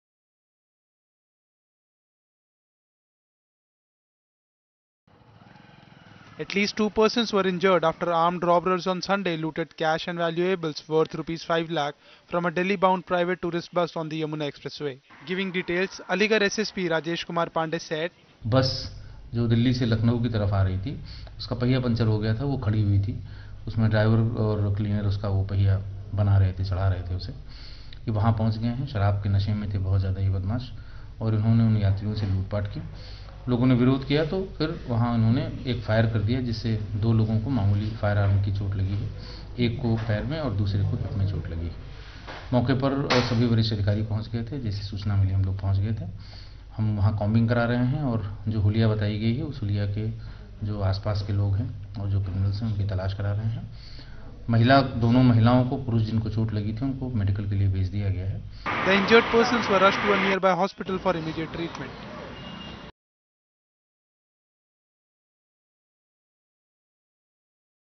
Two injured as armed robbers loot bus on Yamuna Expressway - ANI News
Aligarh, Jan 22 (ANI): At least two persons were injured after armed robbers on Sunday looted cash and valuables worth Rs. five lakh from a Delhi-bound private tourist bus on the Yamuna Expressway. Giving details, SSP Rajesh Kumar Pandey said the incident took place when the bus took a halt after one of its tyres got punctured, adding that they also opened fire when passengers retaliated to their robbery act. The injured persons were rushed to a nearby hospital for immediate treatment.